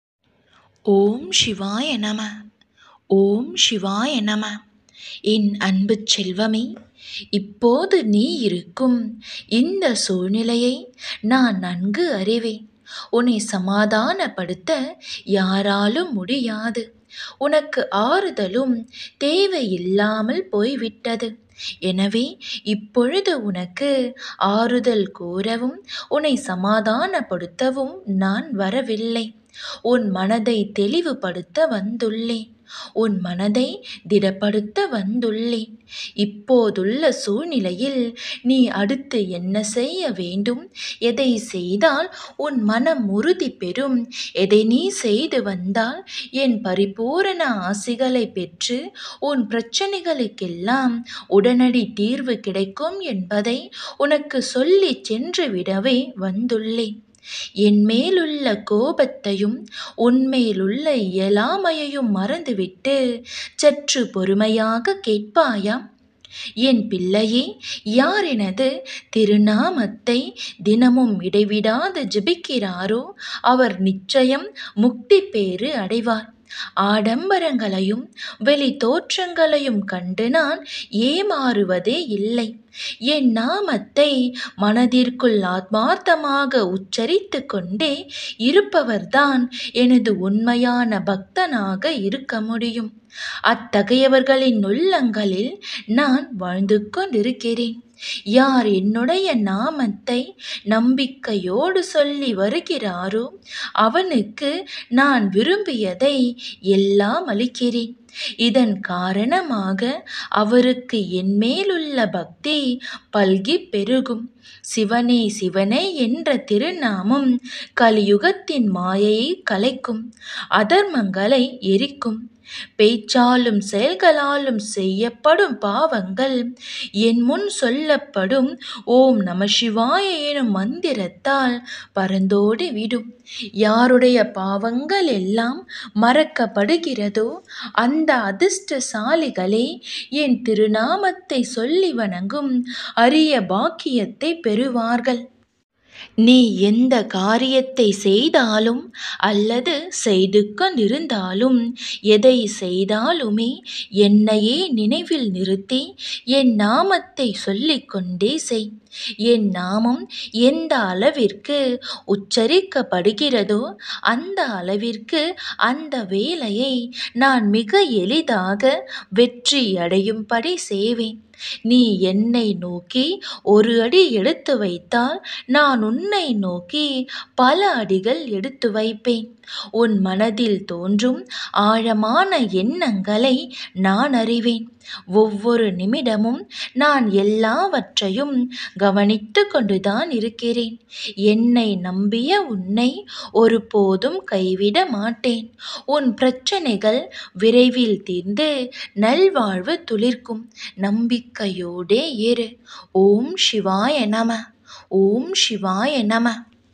உன்னுடைய பாவங்கள் அனைத்தும் மன்னிக்கப்படும் இதை கேள்/ Sivan motivational
SIVAN  motivational speech for successful life

ANBE SIVAM channel helps you to connect with murugan every day with motivational speech to get a positive vibes for improving your well-being ,wealth & health.

With love-